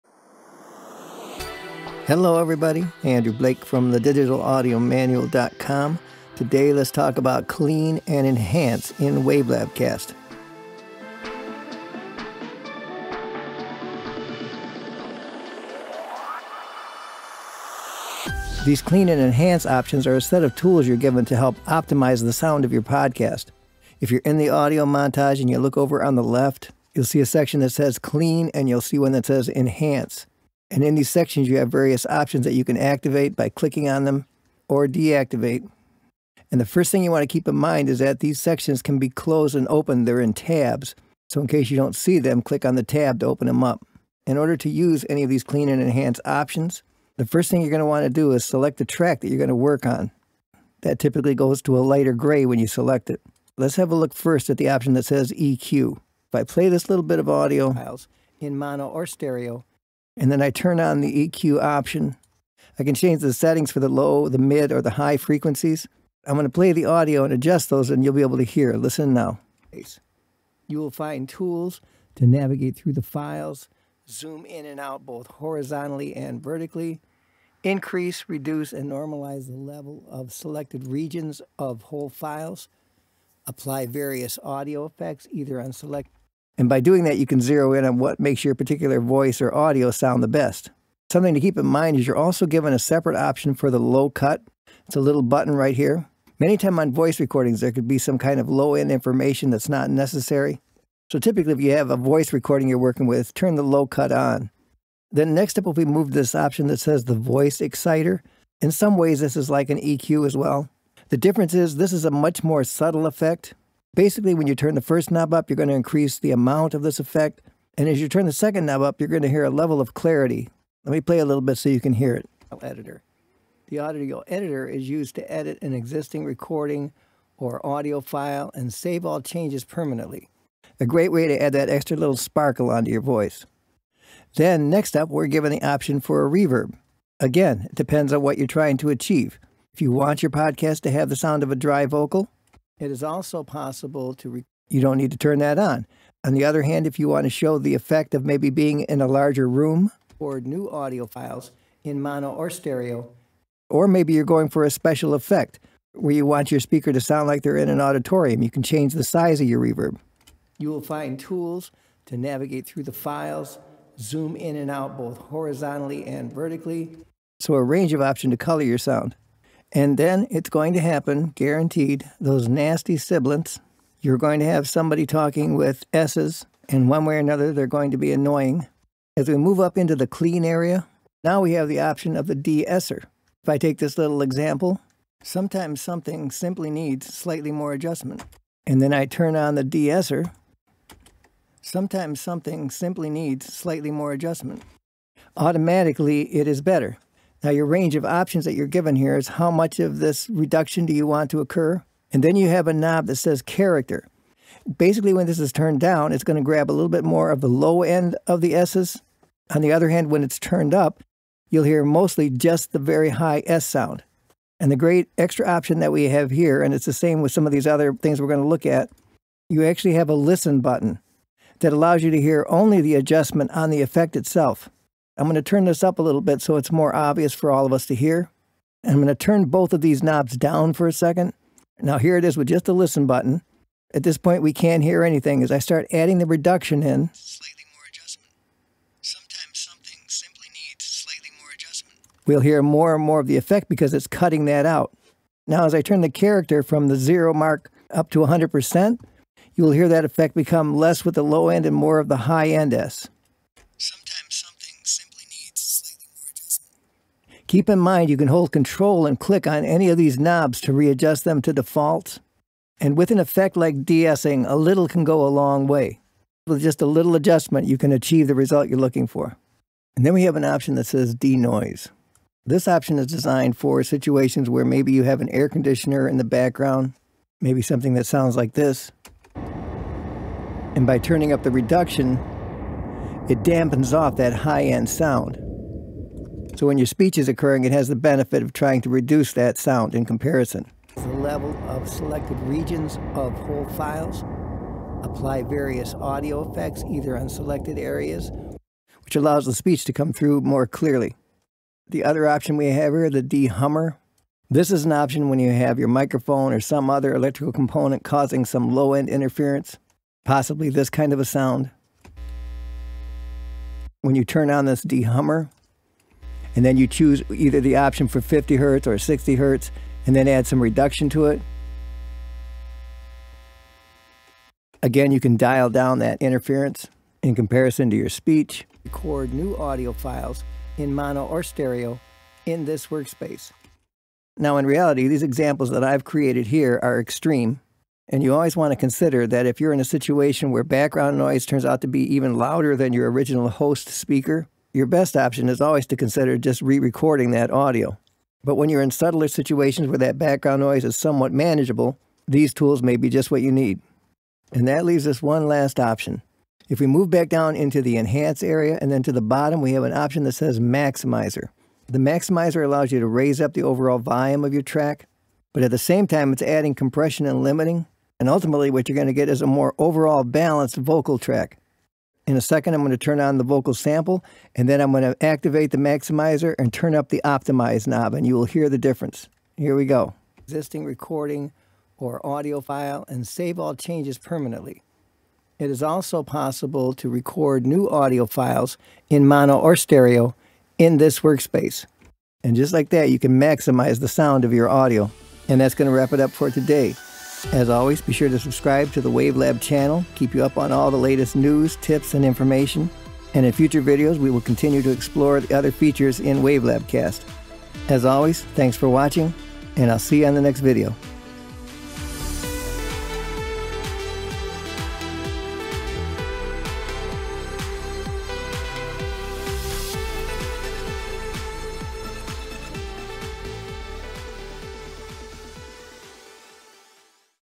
Cleaning and Enhancing Your Recordings | WaveLab Cast Tutorials
This WaveLab Cast tutorial examines how to clean and enhance audio using the tools in the Audio Montage to optimize the sound of your podcast. You can use EQ to adjust the low, mid and high frequencies, normalize levels, enhance voice recordings by applying effects to make a voice sound its best and increase clarity, use the de-esser to reduce sibilance, make background noise less intrusive with the denoise and dehummer effects, add compression and limiting plus much more.